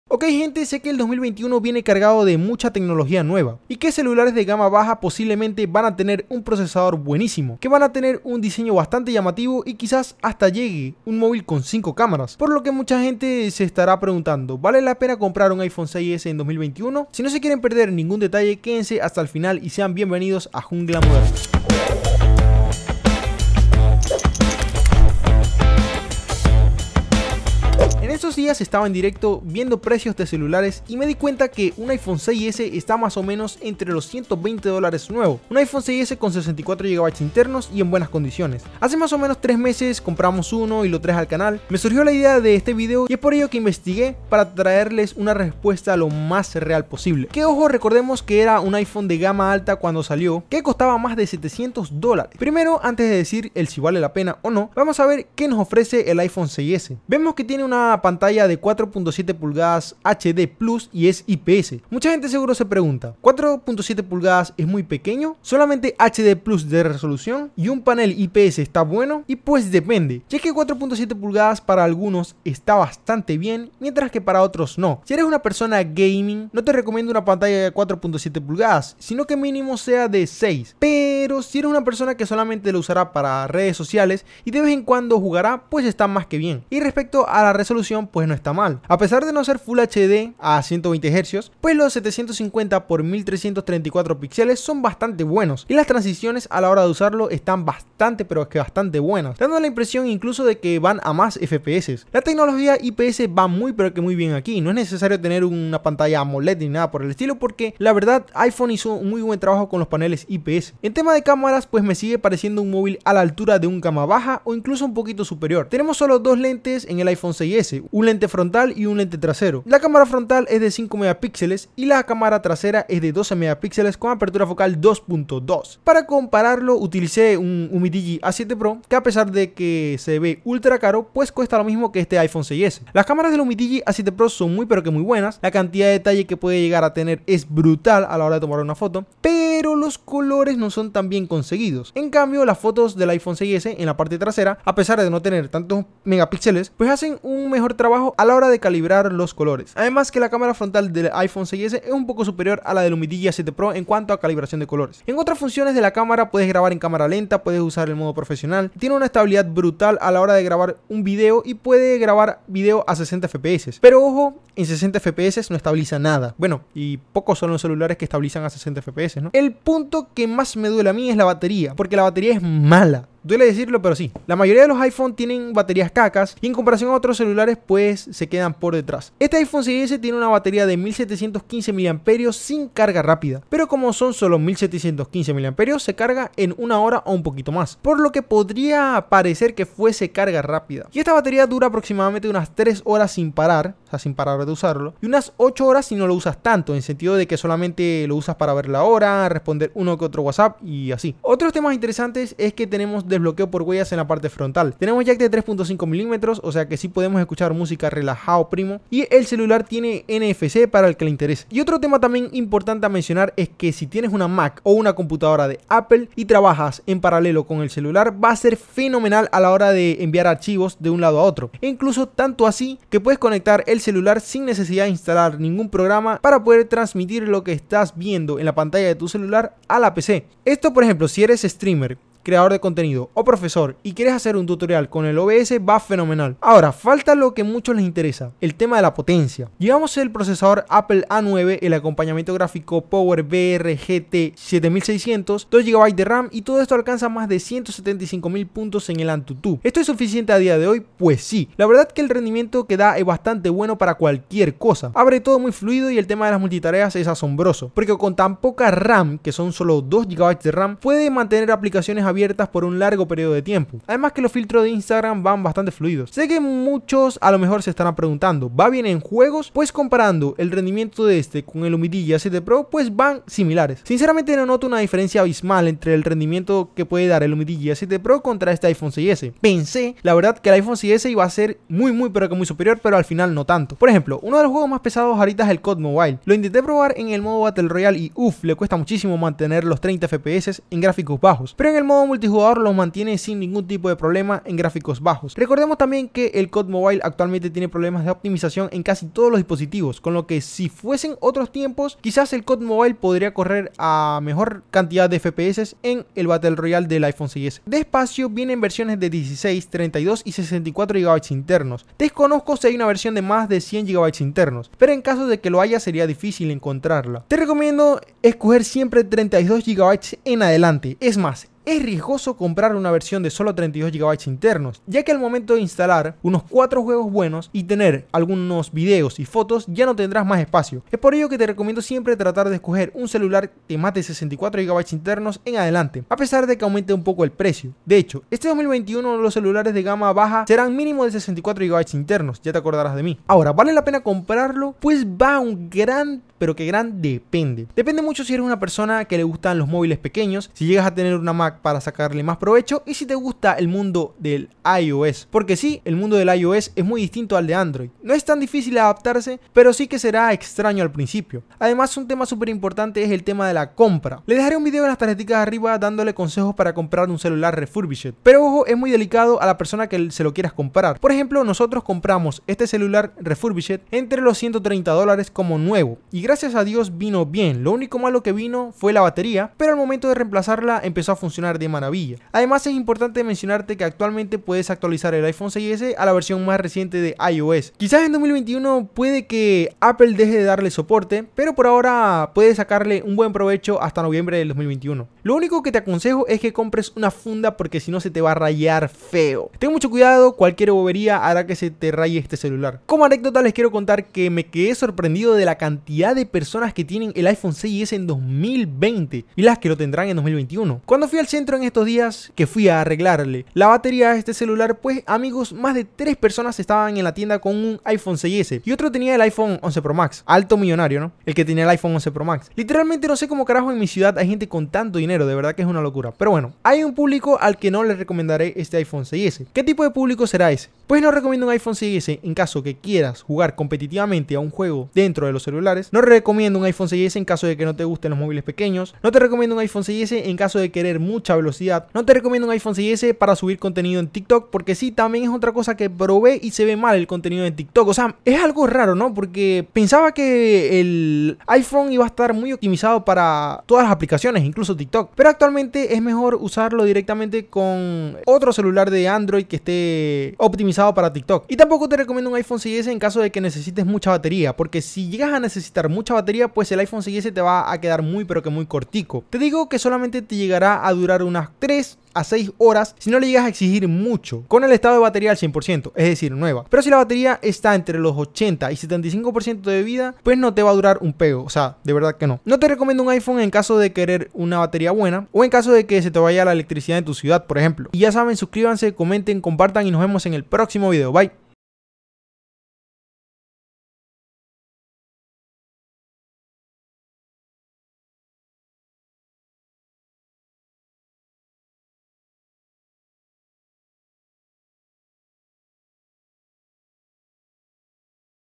¿VALE la PENA el iPhone 6s en 2021?
El iPhone 6s es un tremendo celular que fue muy comprado cuando salió, incluso mucha gente hoy lo sigue comprando y lo más seguro que la gente lo siga comprando, por lo que seguramente surga la duda: ¿Vale la pena el iPhone 6s por su precio? Pues si tienes esa duda te invito a ver este video hasta el final.